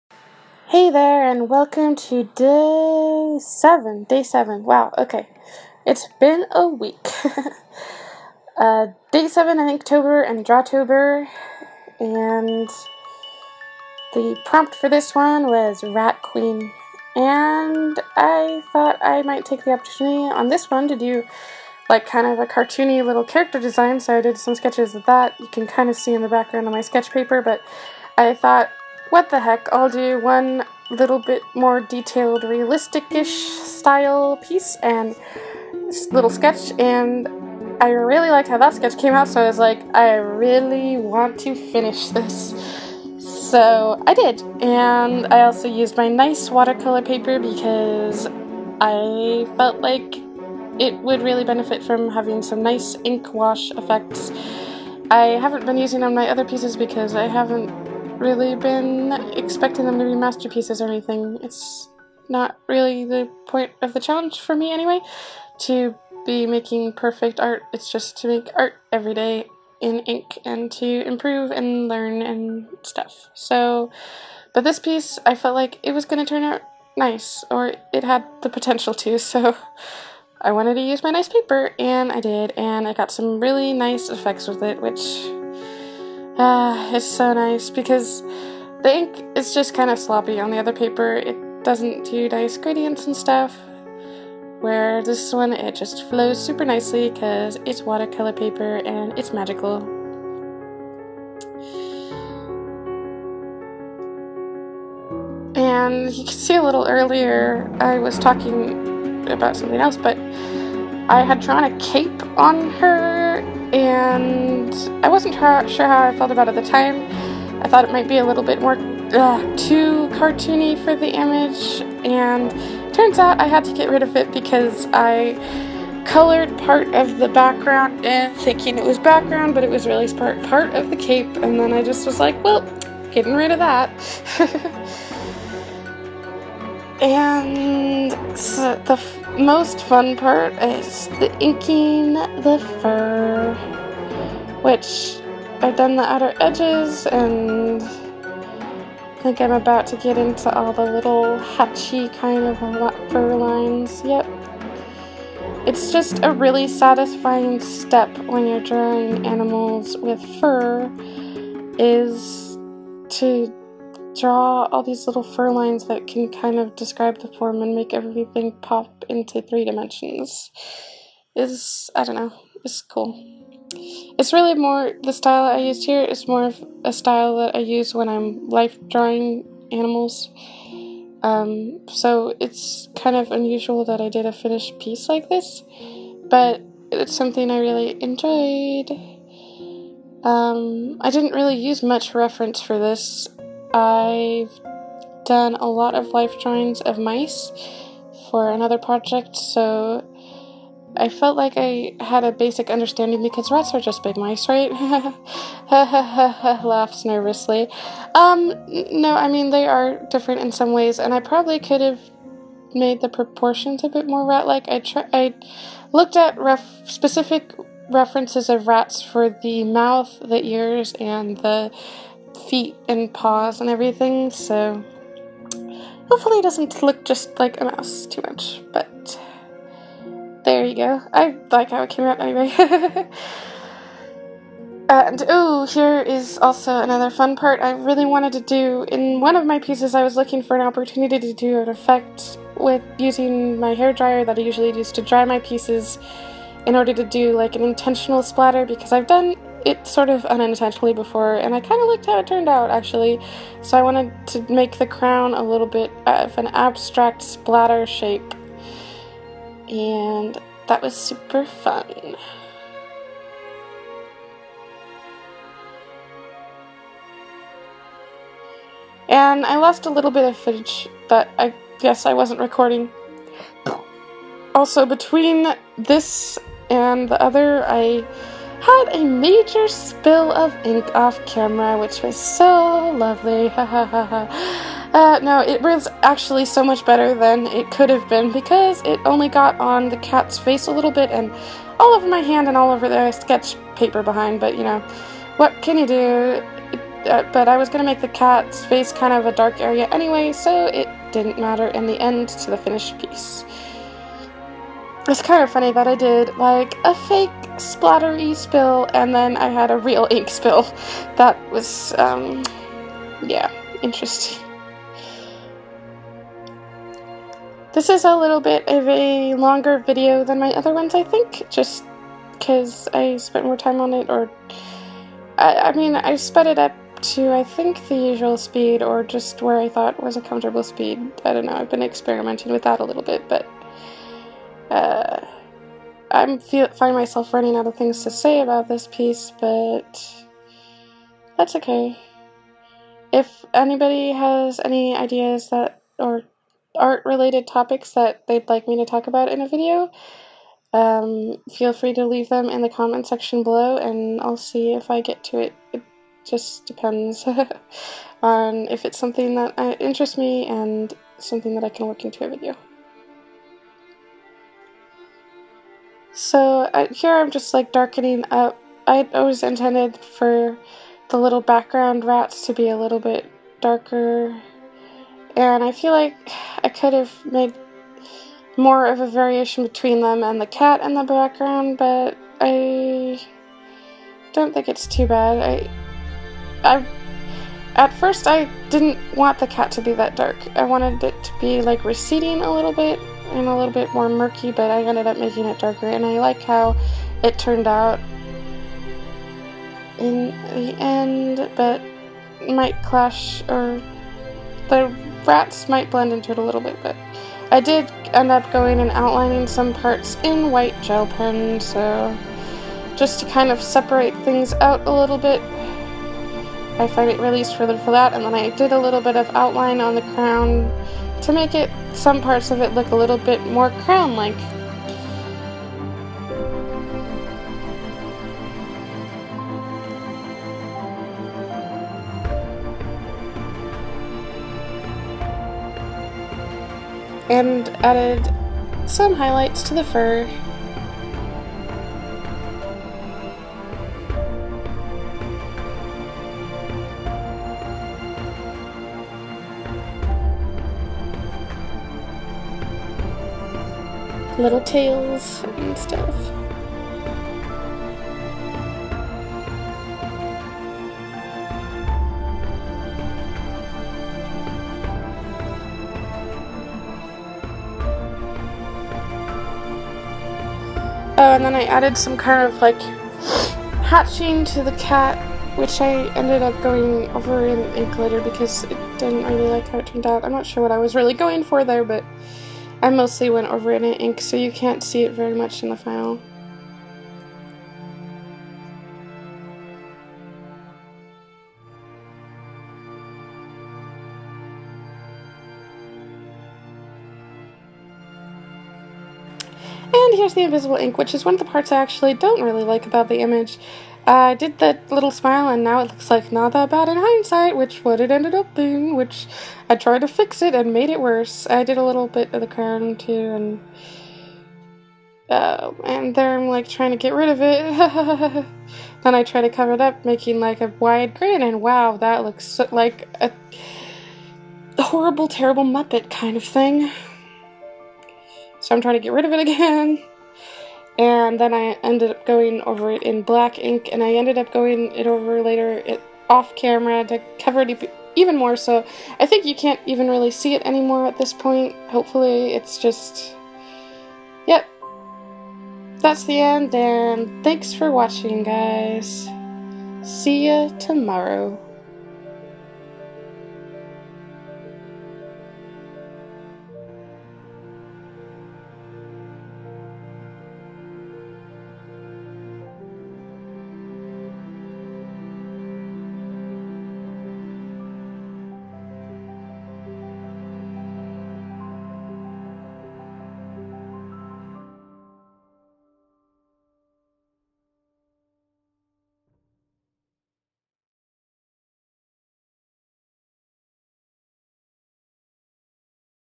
Inktober + Drawtober 2017 Day 7 - Rat Queen
Apologies again for the poor video quality, tomorrows *should* be better.

Materials used:
Arches Cold Press Watercolor Paper
Deleter Manga Starter Set
India Ink
Copic marker
White gellyroll pen
Invisible Ink